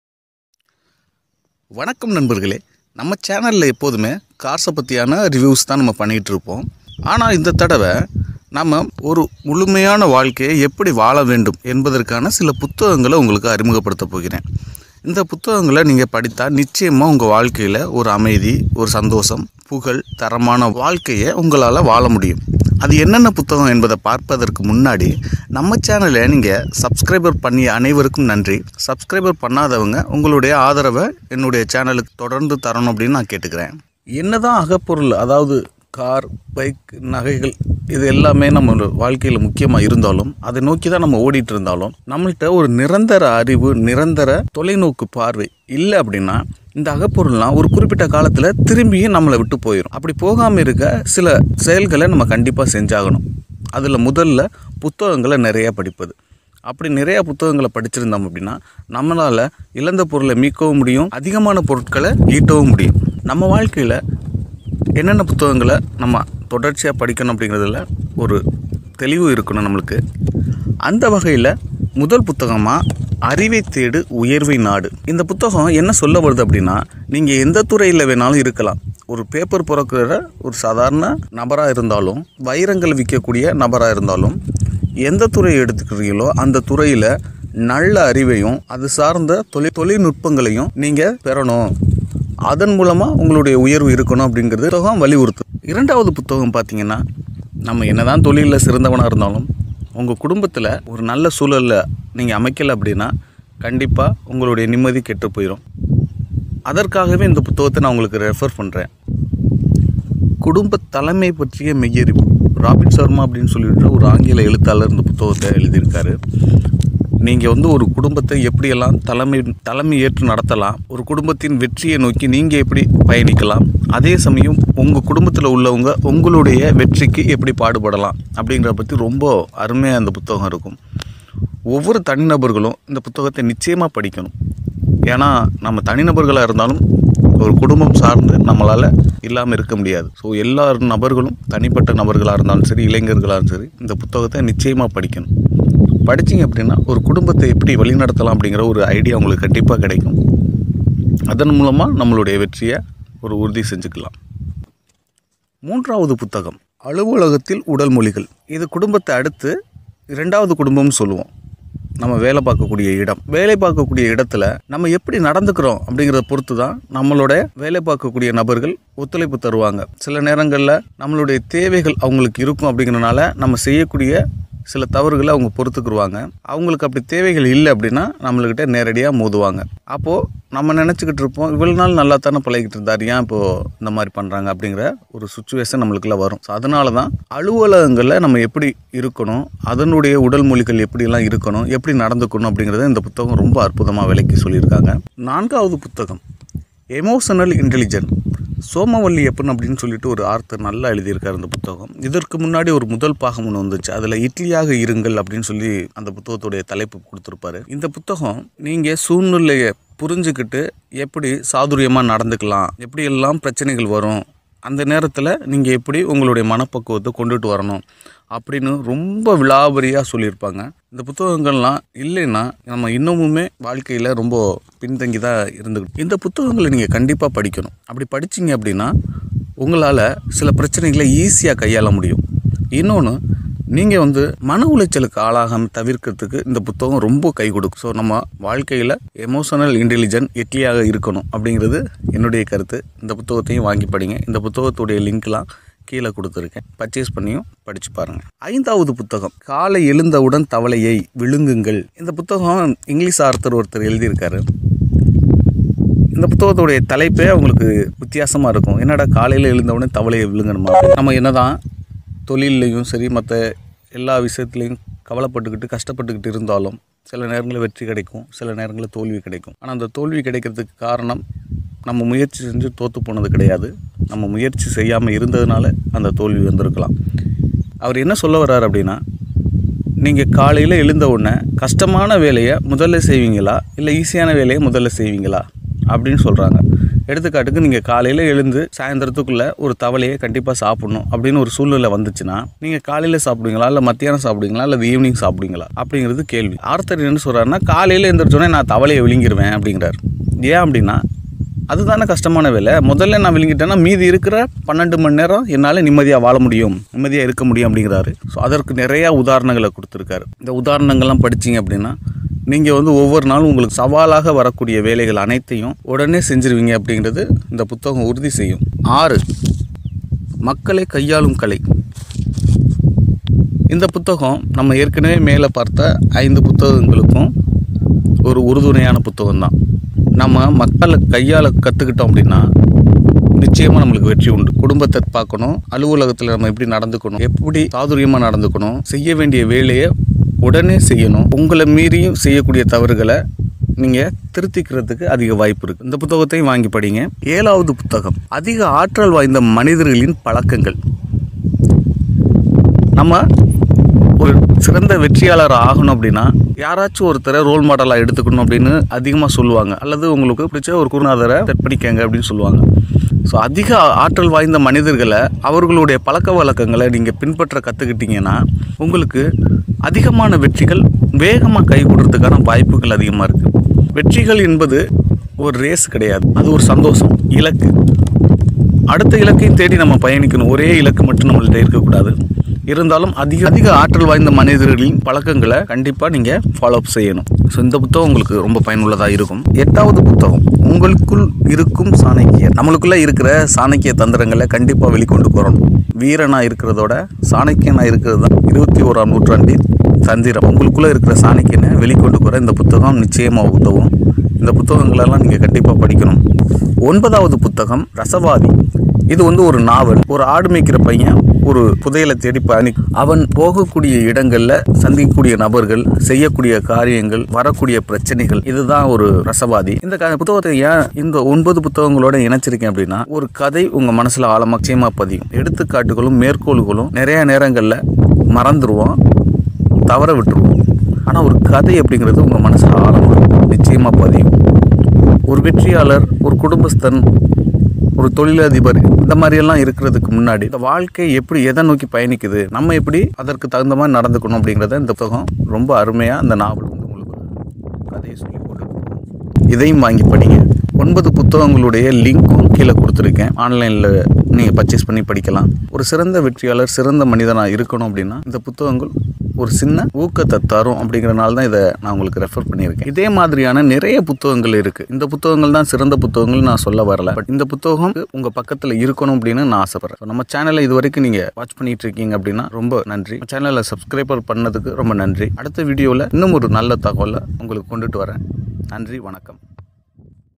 சிறந்த வாழ்க்கைக்கு 9 புத்தகம்
குடும்ப தலைமை பற்றிய மெய்யறிவு
அலுவலகத்தின் உடல்மொழிகள்
எமோஸனல் இண்டெலிஜென்ஸ்
மக்களை கையாளும் கலை
ஆற்றல் மிக்க மனிதர்களின் 7 பழக்கம்,
காலை எழுந்த உடன் தவளையை விழுங்குங்கள்
உங்களுக்குள் சாணக்கியர்
,ரசவாதி நாவல்